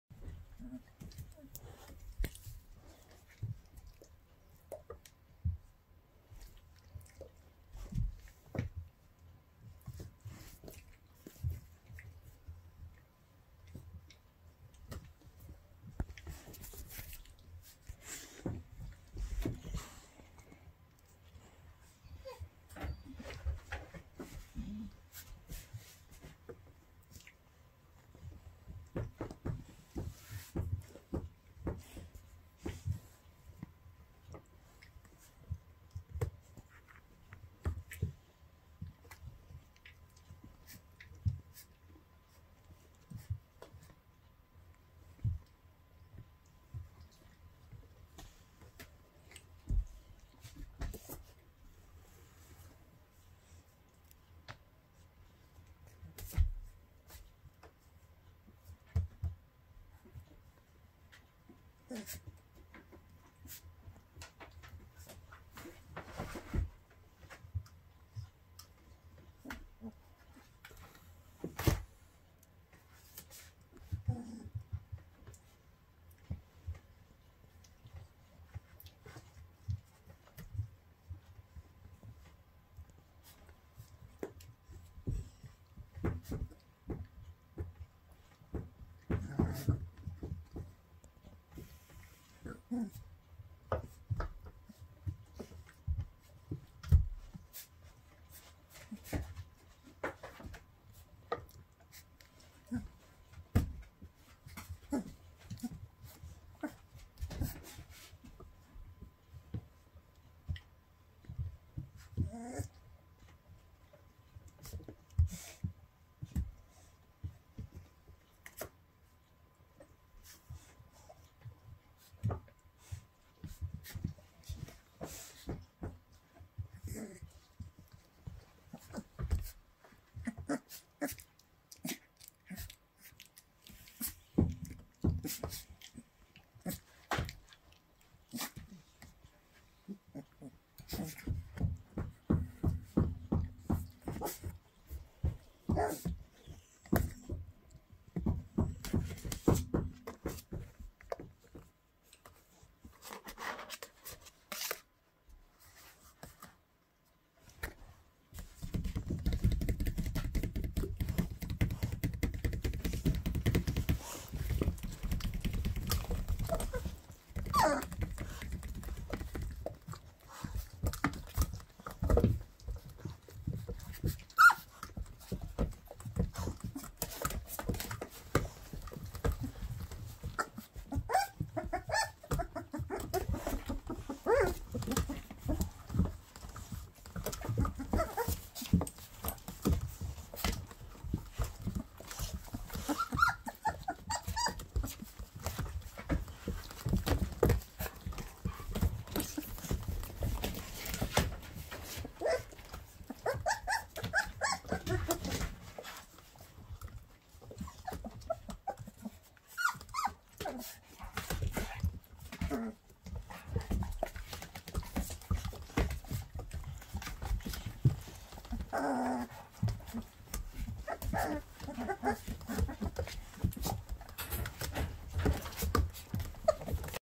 Litter X - Playful Interactions
The pups are more and more interactive with each other the older they get, at a little over three weeks old, they can see and hear and their personalities are starting to emerge. As you can see throughout the video, the pups are starting to play with each other by mouthing and pawing at a littermate. They also get excited when they see momma Skylar peeking in for a visit. -- Video taken 12/29/19 @ 10:32 am